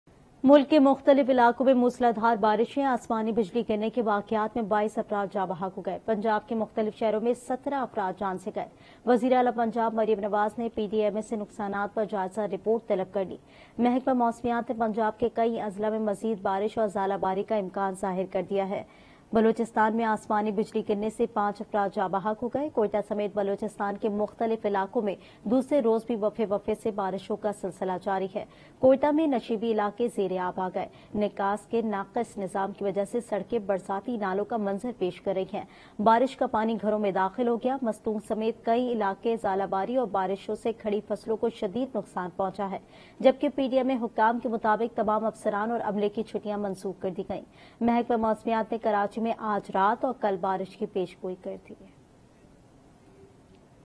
Heavy rains in different parts of the country - Aaj News
Heavy rains in different parts of the country - 14 April, 2024 - Aaj News

Spot Light With Munizae Jahangir:
shorturl.at/gwFU6